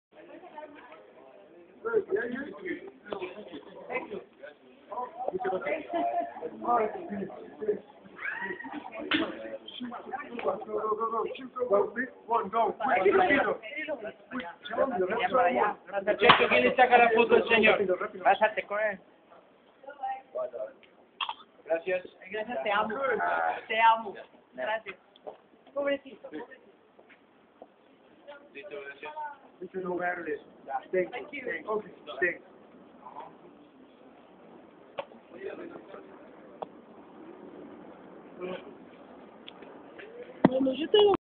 STEVEN TYLER EN PASAJE POLANCO D.F. 2011
steven tyler haciendo shoping en el pasaje polanco con un sequito de seguridad que no permitia acercarse a el fue un logro haber llegado hasta el. gracias a una persona que grabo el momento y me lo paso